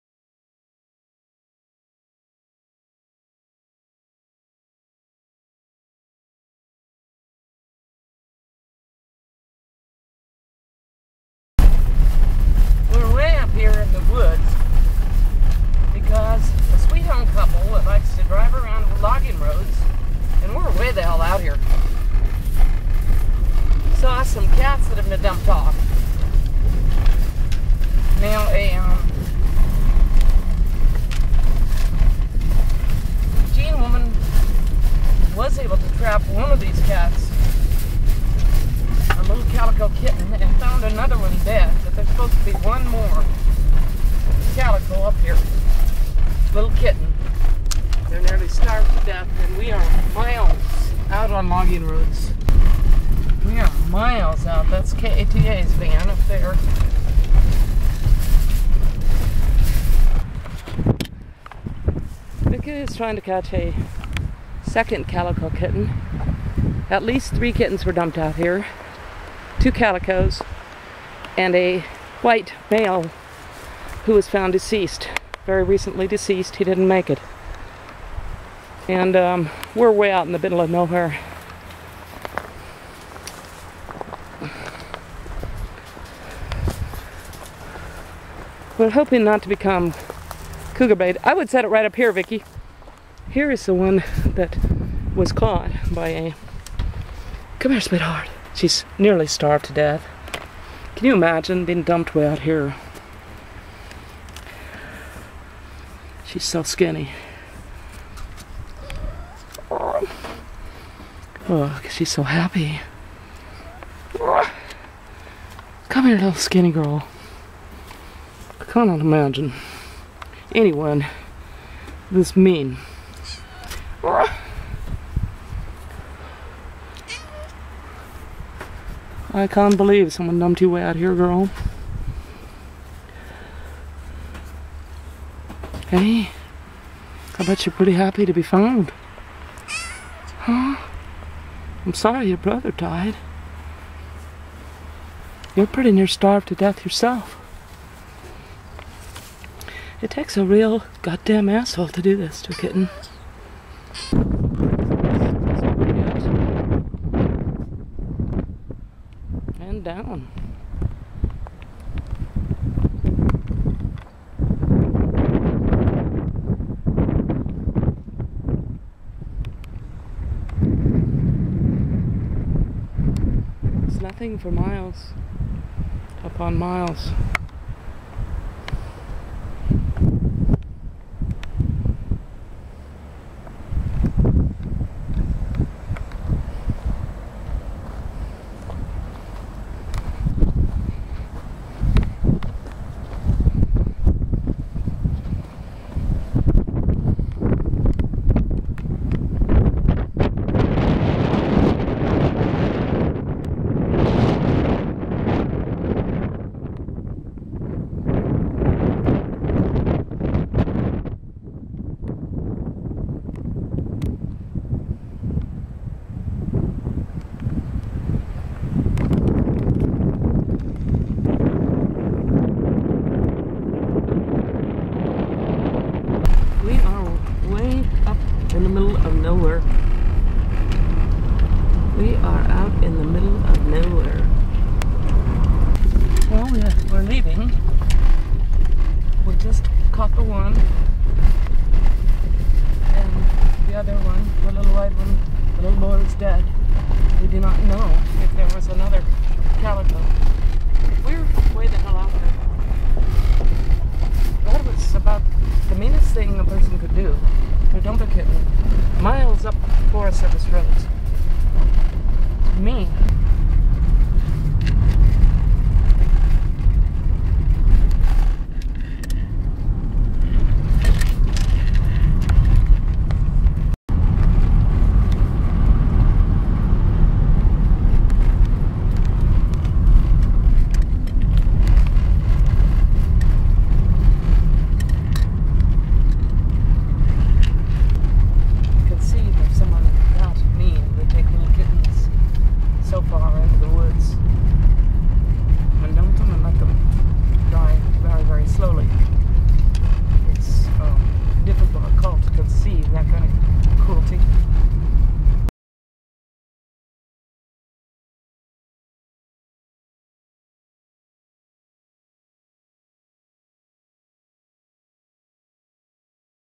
Who Abandoned This Kitten in the Wilderness Above Marcola, Oregon?
An old couple was out driving logging roads, as they often do, and, near an area frequented by teen partiers and off road four wheelers and even hang gliders, they saw two white calico kittens by a mud hole created by off roaders.  They called Eugene cat rescue groups hoping someone would go help them.  A Springfield woman and her children rented a trap and went up.  They got lost at first.  The roads are confusing and rugged.  They were able to trap one of the kittens, a starving sweet calico and found another dead.  There may be a second calico in the woods still.  What kind of a coward/monster dumps kittens so far out in the wilderness to die slowly?